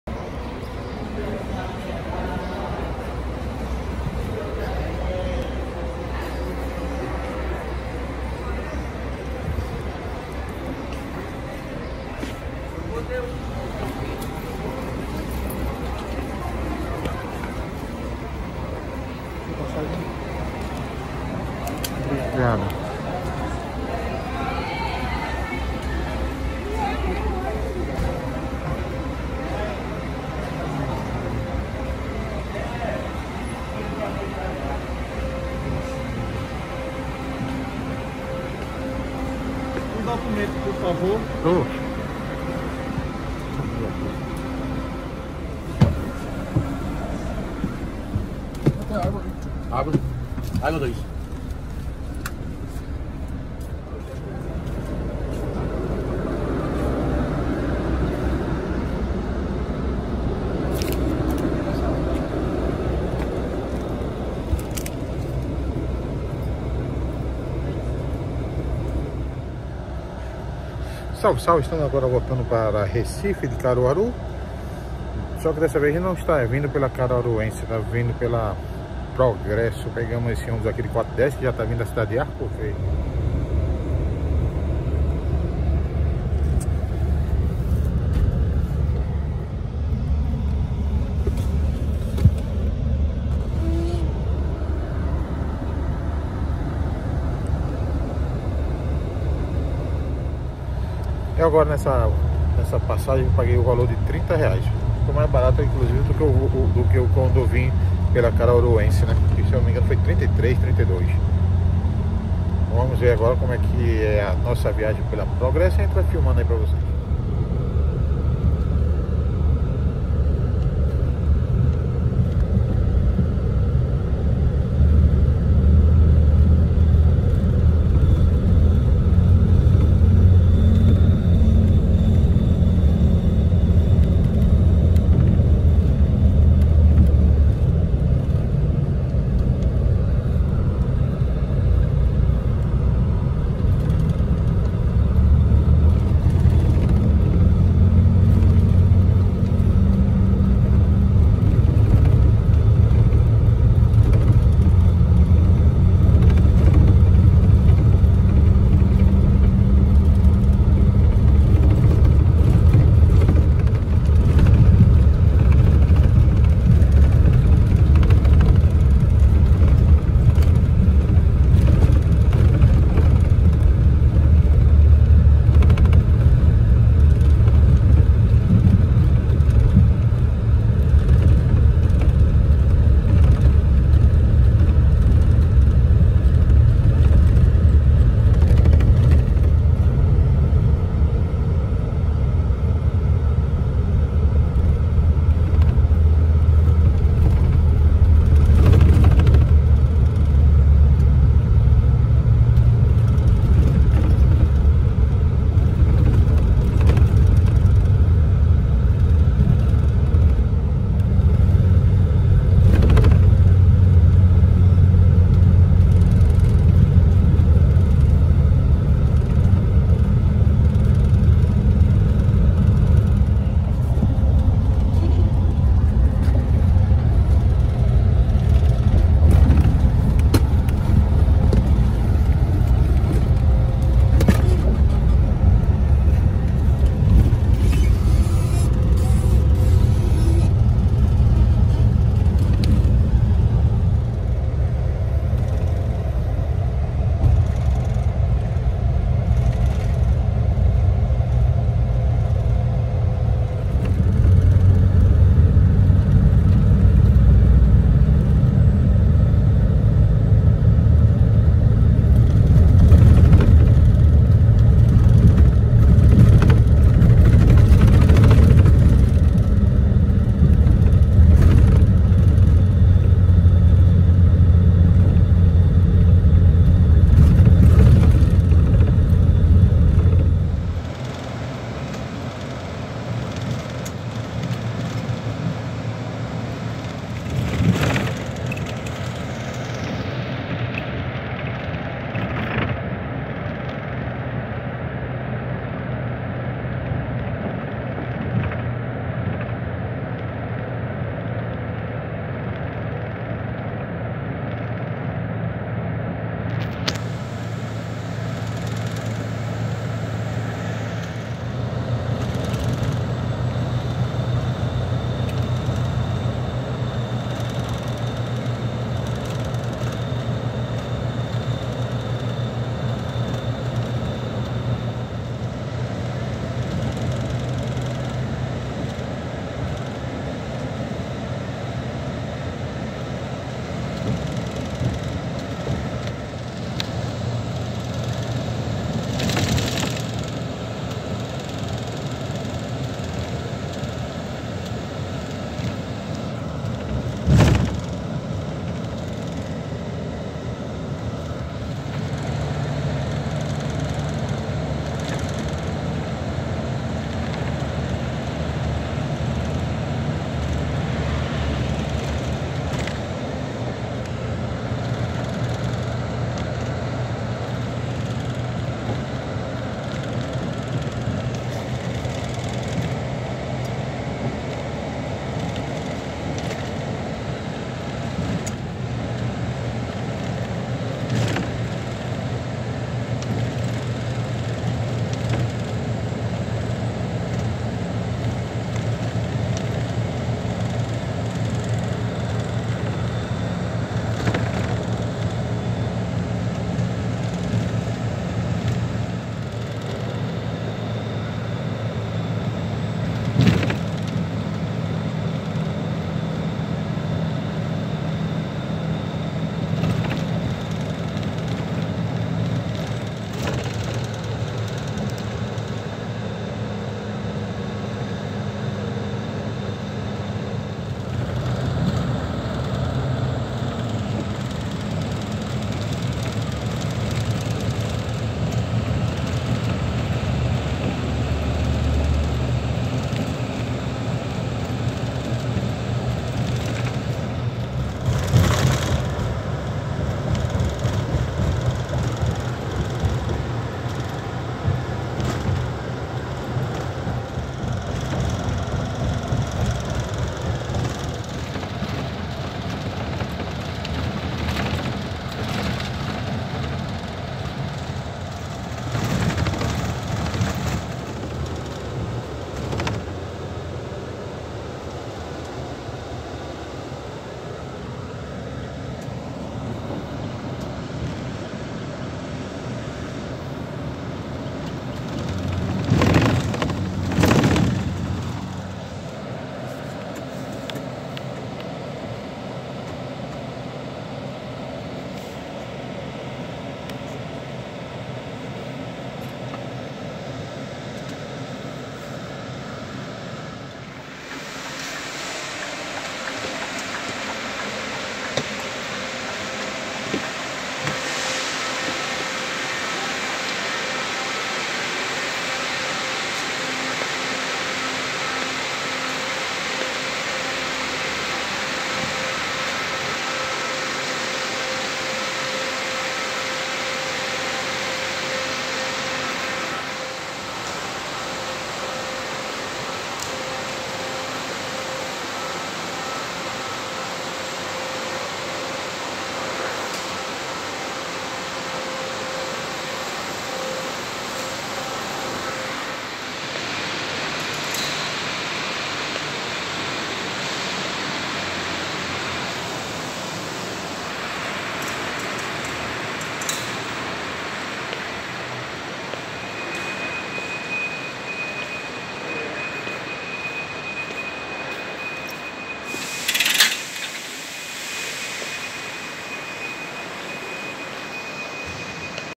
Trip Report de Caruaru para Recife pela Progresso
Voltando com a progresso de Caruaru até Recife